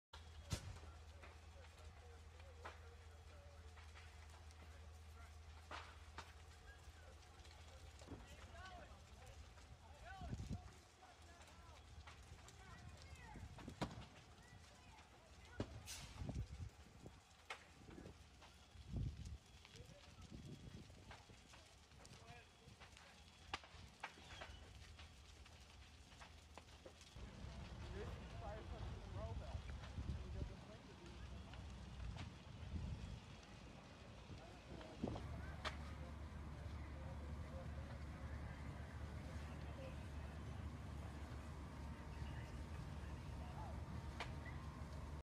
Pennsylvania home explosion leaves four dead, three hospitalized
Multiple houses caught fire after an explosion on Rustic Ridge Drive and Brookside Drive in Plum Borough.